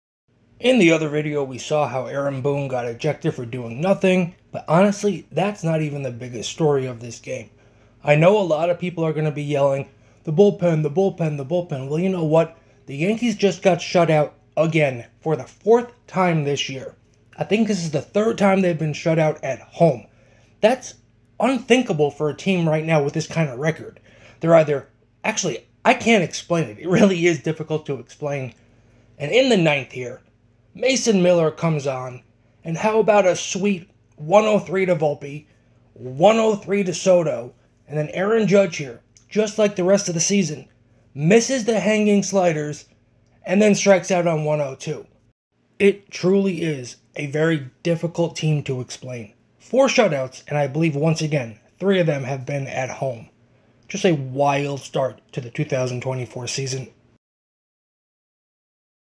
Oakland Athletics just shut out the Yankees!! Mason Miller is filthy
Oakland Athletics vs. New York Yankees
Mason Miller throwing 103 to strike out the side in the 9th.
MLB / Baseball
MLB 2024
Grunttalksmlb.com
Altered MLB Content  Voice Over alterations